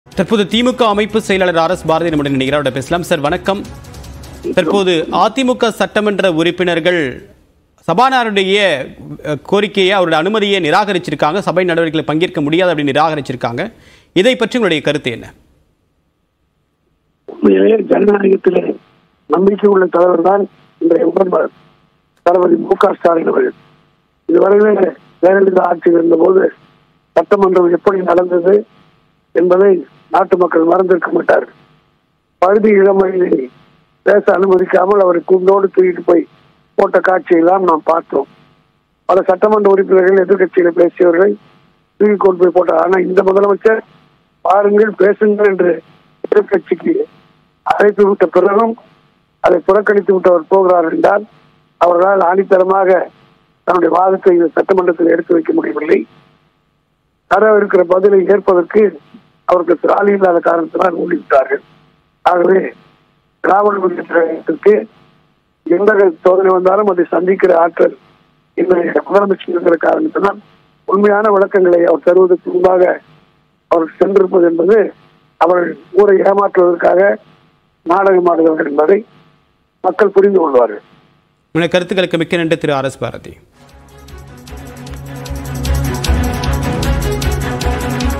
"நம் முதலமைச்சர் வாருங்கள்,பேசுங்கள் என்று அழைத்துவிட்ட பிறகும் அதை புறக்கணித்த அதிமுக" | ADMK
"நம்ம முதலமைச்சர் வாருங்கள், பேசுங்கள் என்று அழைத்துவிட்ட பிறகும் அதை புறக்கணித்துவிட்டு போறாங்க" - விமர்சித்த ஆர்.எஸ்.பாரதி  | ADMK | DMK | TN Assembly

list=PLZjYaGp8v2I8Jl8PfHo2MQdWKs42-15cX